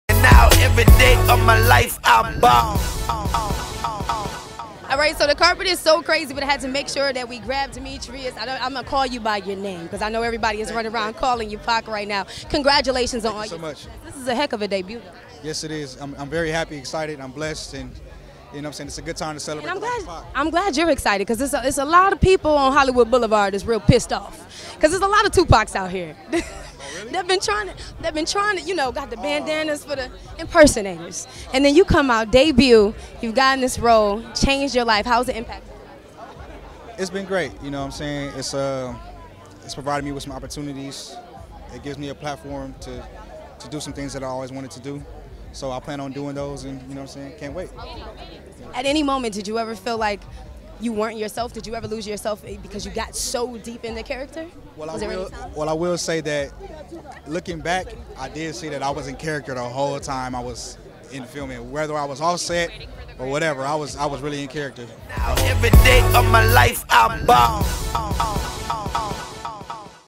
Baller Alert- Dimitrius Shipp Jr. Talks Playing Tupac in "All Eyez On Me"
Lead actor, Dimitrius Shipp Jr. talks playing Tupac.